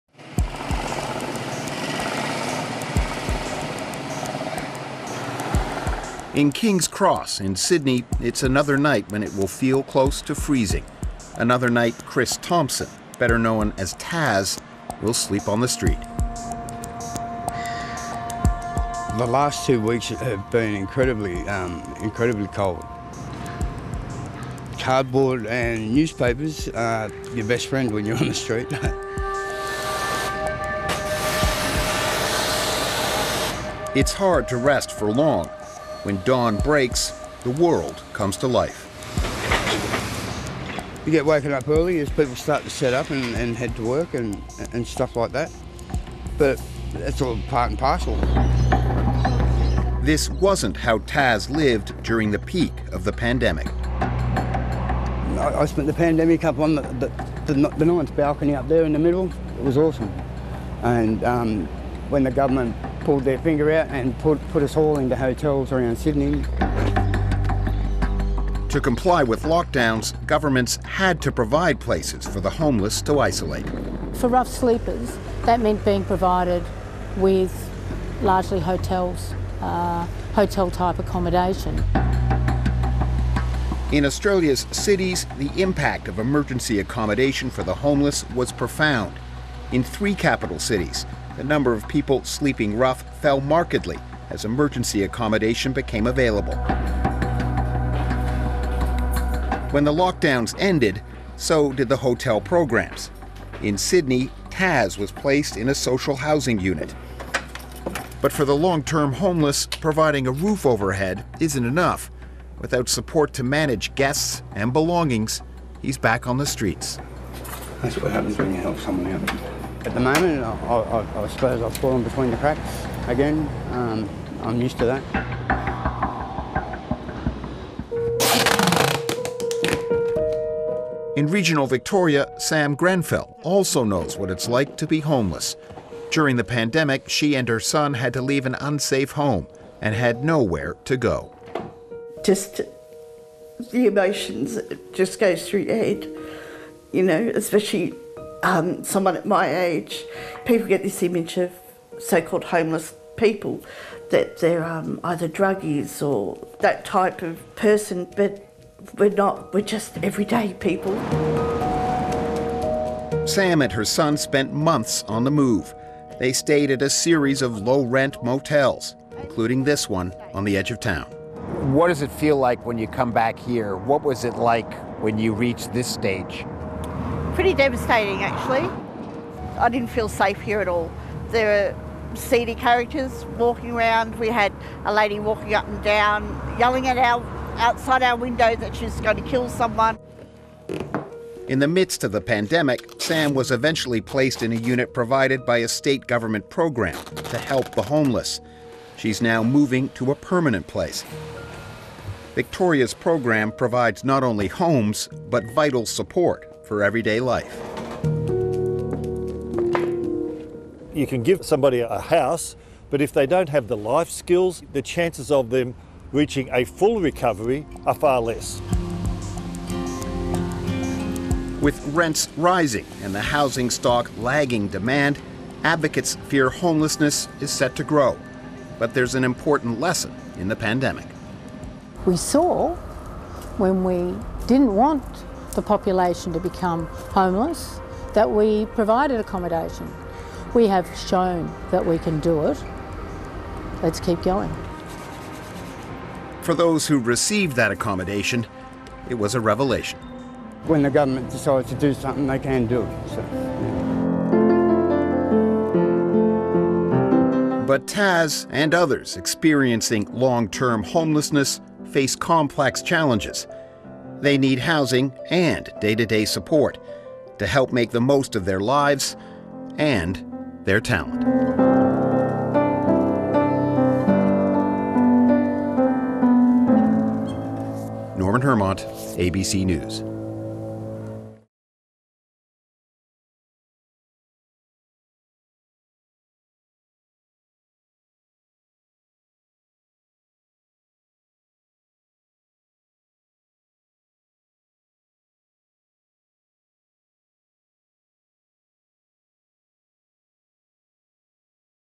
Governments housed homeless in hotels during lockdowns — but many are back sleeping rough | ABC News
As the winter cold settles in, homelessness is once again hard to ignore. During COVID pandemic lockdowns, thousands of rough sleepers and those in shelters were put into hotels to adhere to social isolation rules.
But many of those people are now back on the streets.
Norman Hermant has this special report.